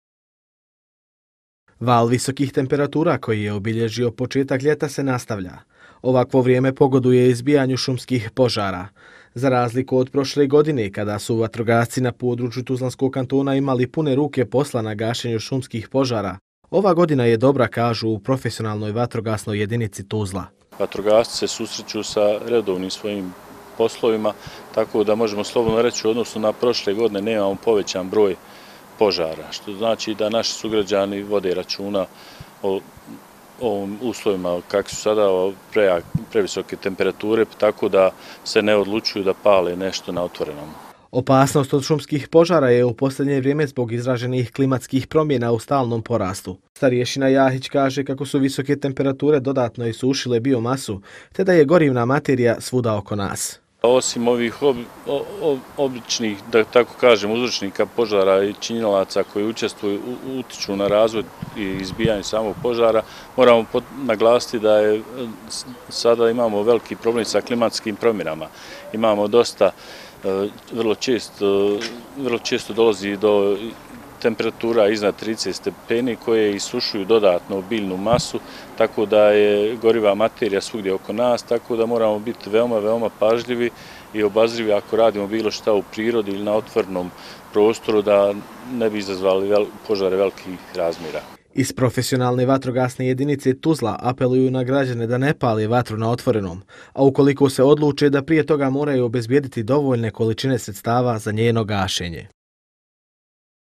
OPASNOST OD LJETNIH POŽARA 09 08 2021
Na krajnjem jugu Bosne i Hercegovine, samo u protekla 24 sata registrirano je dvadeset požara. Vijesti o požarima na jugoistoku i jugu Europe u ljetnim mjesecima nisu ništa novo, ali ove godine su novost njihov intenzitet i broj. Vatrena stihija je već odnijela ljudske živote. A opasnost ne jenjava. U Profesionalnoj vatrogasnoj jedinici Tuzla pitali smo kakva je situacija na području TK i kako spriječiti požar.